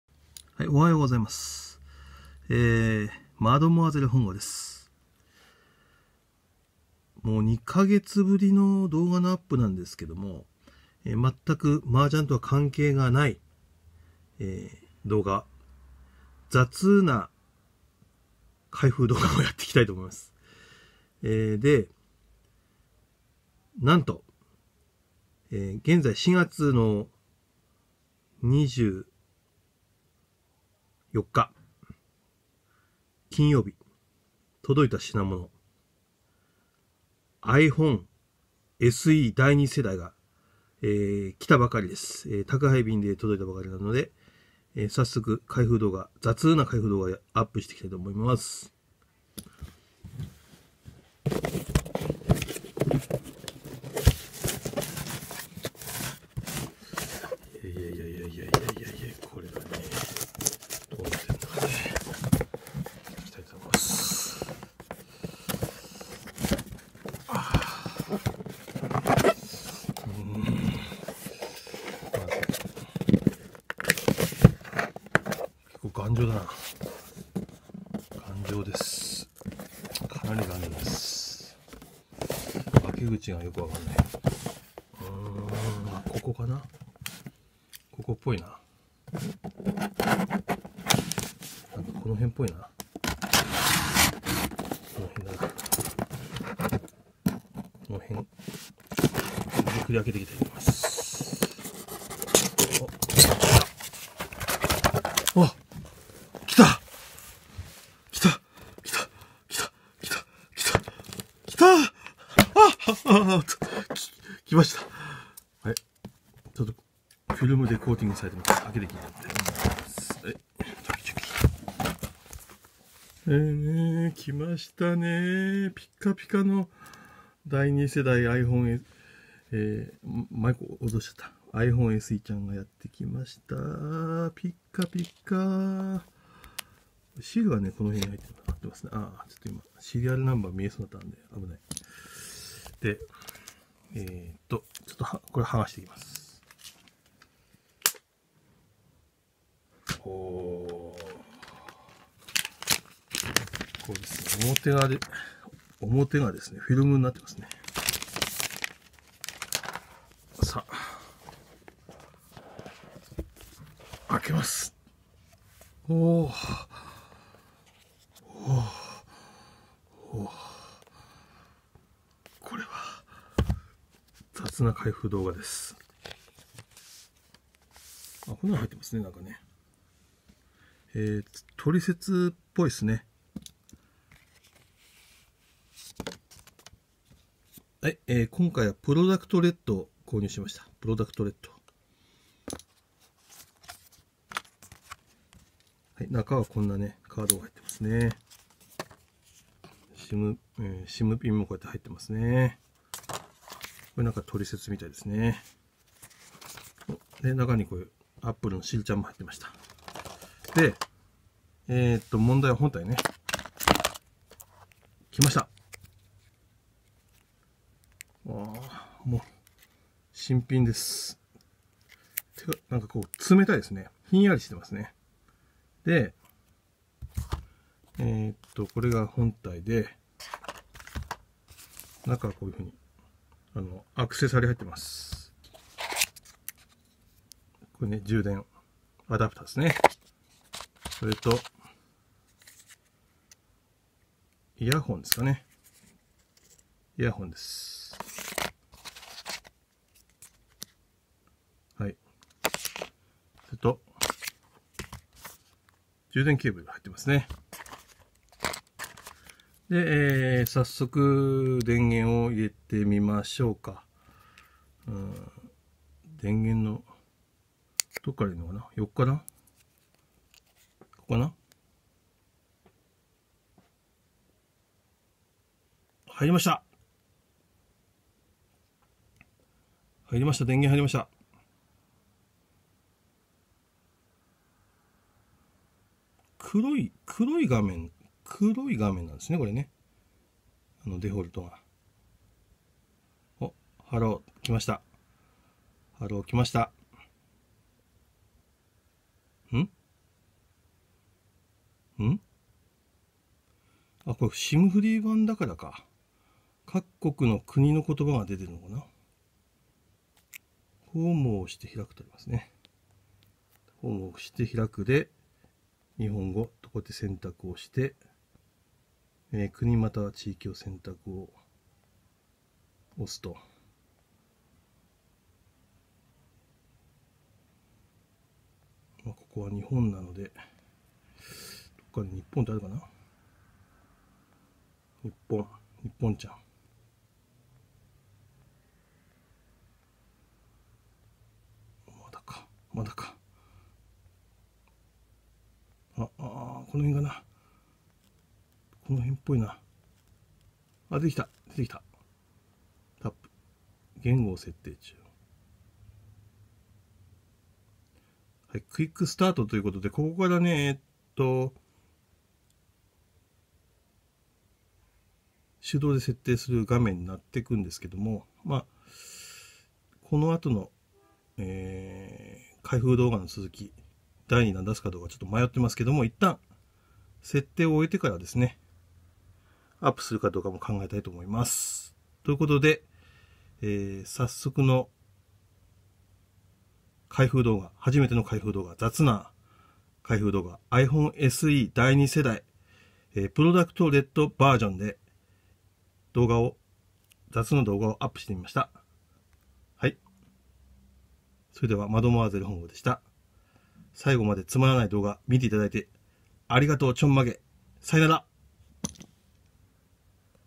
★雑な開封動画＃01★iPhone SE 第２世代 SIMフリー★プロダクトレッド
4/24 金曜日 9:00にクロネコヤマト便で到着したばかりです！(^^)
★新しいiPhone SE — Apple★